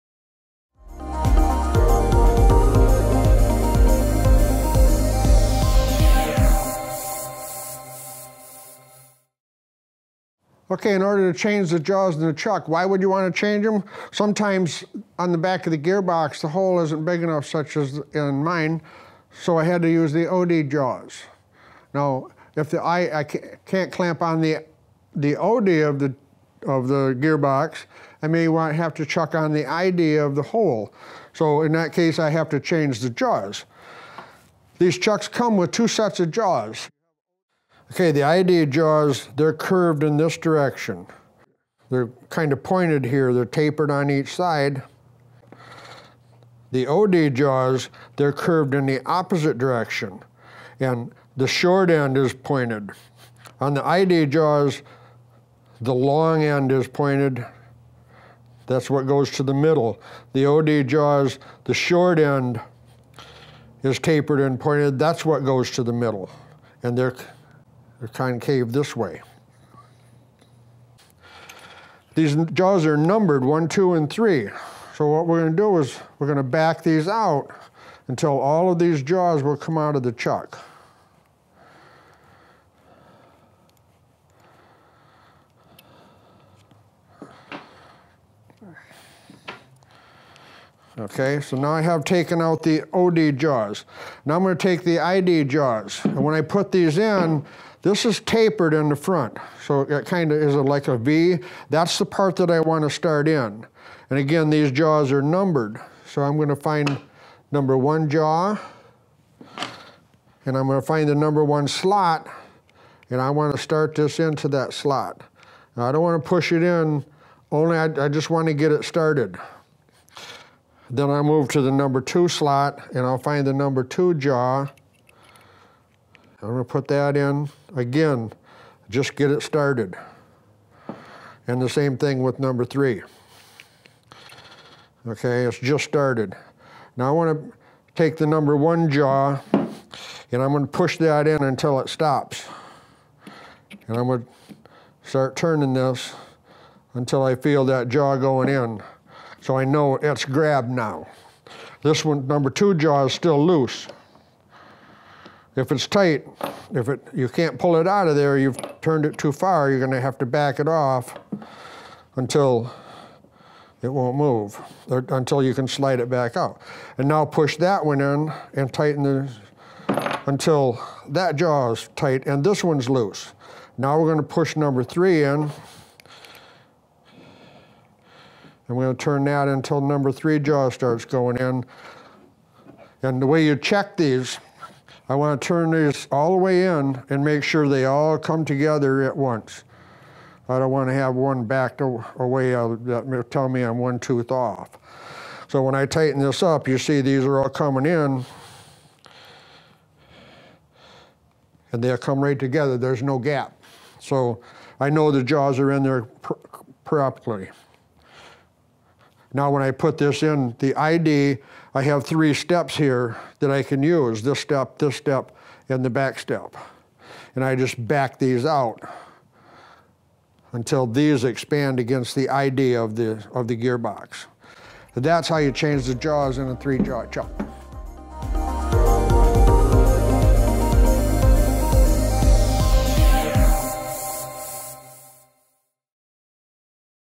L-705 Bore System - Extruders - Changing the Jaws in the 3-Jaw Chuck
HLI Training video showing how to change the "jaws" of the A-705 Self-Centering Chuck fixture from clamping in the ID of an extruder gearbox counter-bore to clamping on the OD for those times when the counterbore is too small to use the chuck's ID "jaws".  This requires replacing the ID chuck "jaws" with the OD jaws.  It's a simple procedure but does require specific steps.